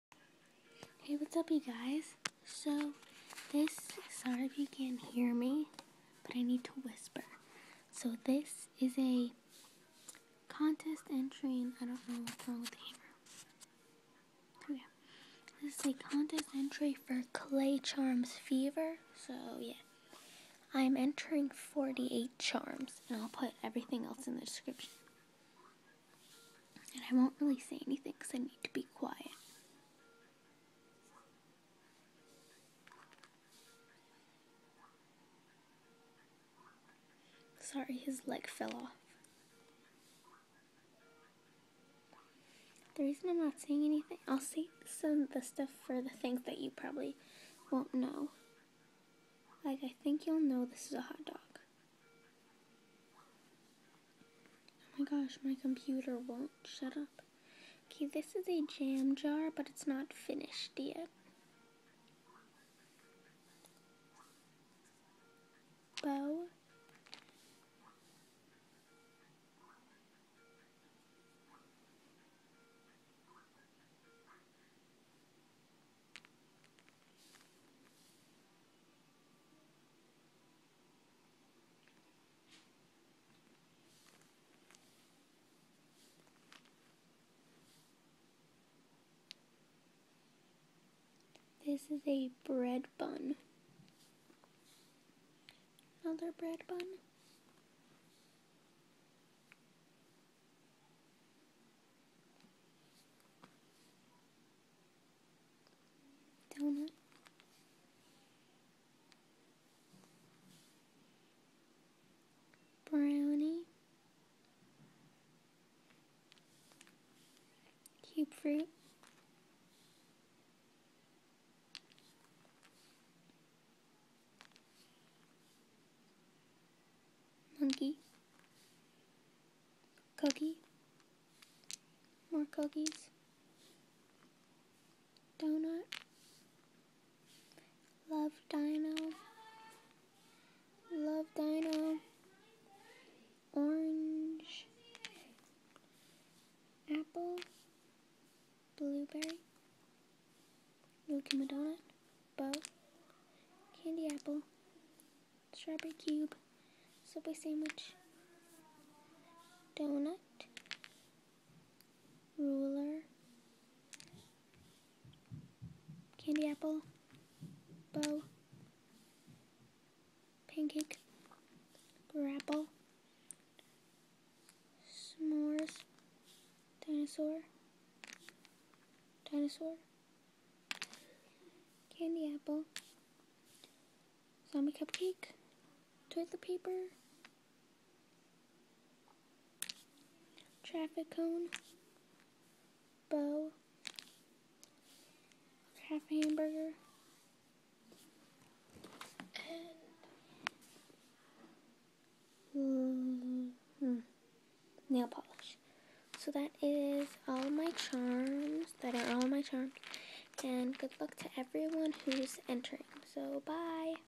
CONTEST ENTRY CLAYCHARMFEVER
i am entering 48 charms i am 12 and i hav been craftimg dor bout a month or 2